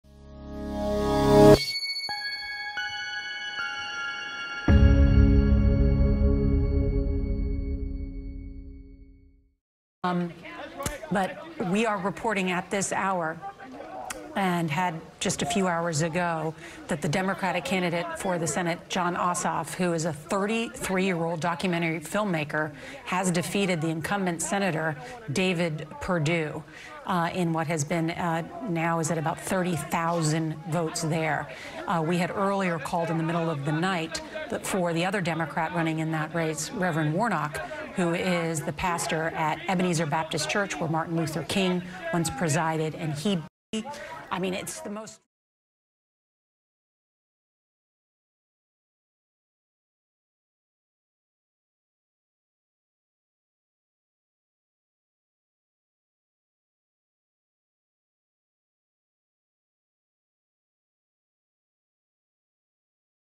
CBS News calls second Georgia senate seat for Jon Ossoff
Amidst a flurry of other breaking news, CBS projected that Jon Ossoff would win the second of two Georgia senate seats. See how other other networks covered the news